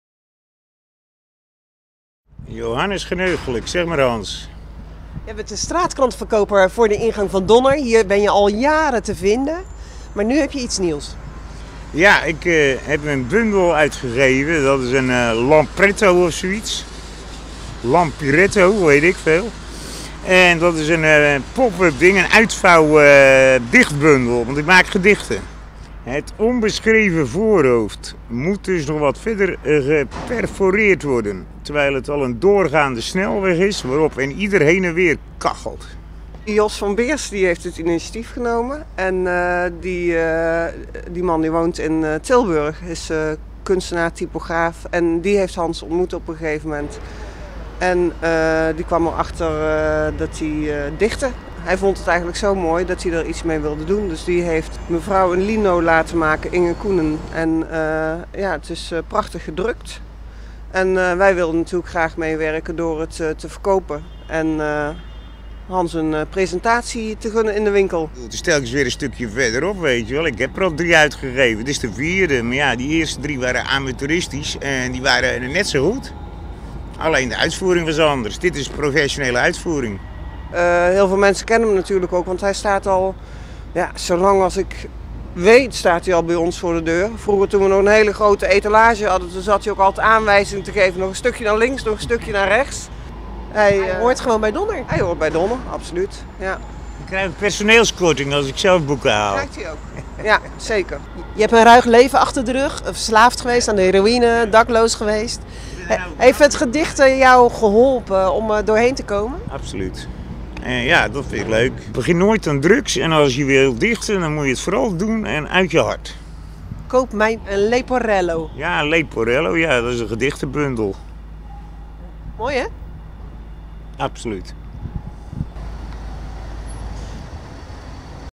Straatkrantverkoper: 'Dichten hielp mij op straat overleven'
Hans Geneugelijk is dichter. Hij is ook straatkrantverkoper, regelmatig staat hij bij de ingang van boekwinkel Donner in Rotterdam. In eigen beheer gaf hij al drie dichtbundels uit, maar nu is er een officiële uitgave, die ook in de winkel verkocht zal worden. Het dichtwerk &apos;Ik heb een kaartje voor overal en nergens&apos; is in een bijzondere vorm gegoten. Het gaat om een leporello, waarbij de tekst zich letterlijk ontvouwt. Drie kunstenaars maakten deze leporello: beeldend kunstenaar Inge Koenen, ambachtelijk drukker Rozemarijn Oudejans en meestertypograaf Jos van Beers.OverlevenKunstenaar Jos leerde Geneugelijk kennen voor de deur van de boekwinkel en vond zijn verhaal bijzonder. &quot;Ik heb dertig jaar lang op straat gewoond. Het dichten en mijn geloof heeft me geholpen om te overleven&quot;, vertelt de dichter. &quot;Ik sliep in kraakpanden en caravans, heb zelfs een pipowagen gehad.&quot;Stoned van de drugs of niet, Hans kan altijd dichten. &quot;Ik heb altijd opwellingen, ik schrijf op alle blaadjes die ik tegenkom. Toen ik verslaafd was, waren de gedichten wel iets harder van toon. Ik heb een paar keer opgetreden, waarbij mensen over hun nek gingen. Ik vertelde toen over een overleden maat.&quot;